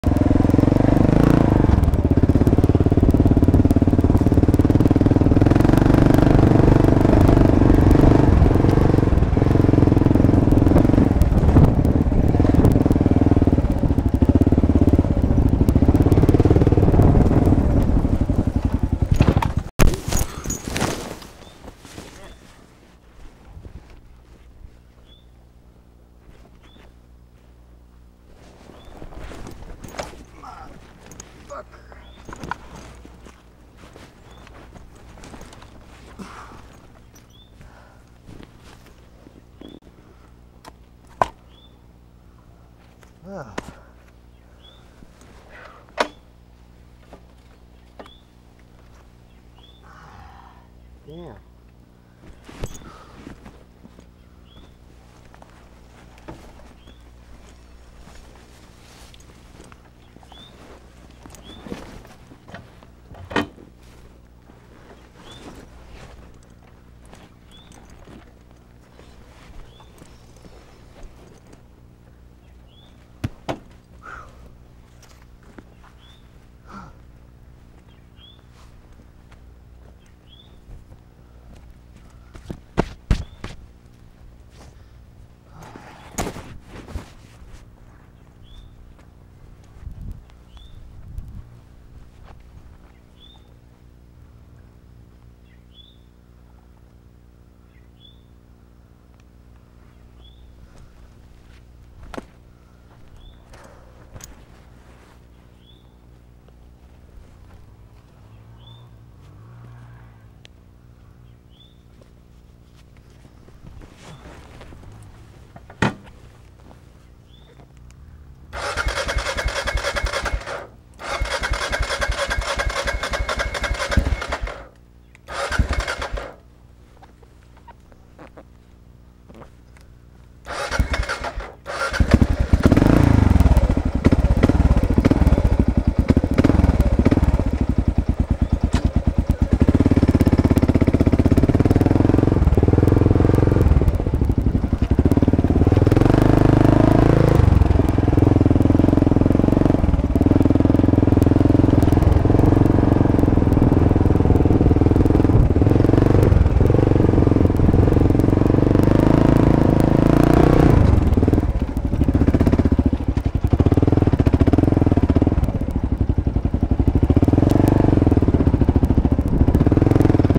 Wipeout in Wixom 7-7-2001
This almost looks like it was on purpose but, its not. I was doing some trail riding in Wixom Michigan and had already ran this trail back and forth several times. So i decided to turn on the helmet cam. It figures that this run was the one where the wheels slipped out from under me.